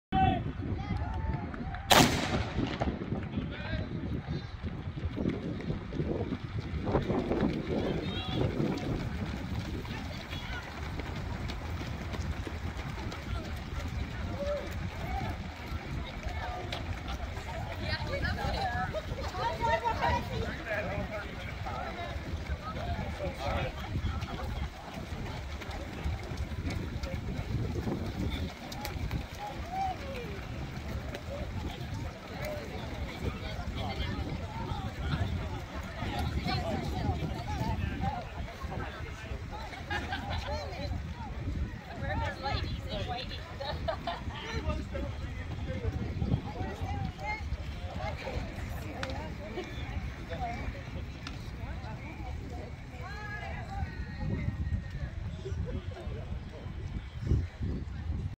WTRC RRRR-2022 by LV
WTRC's Annual October race at Texas Tech Univ. Red Raider Road Race, 5K start video by Larry Vigil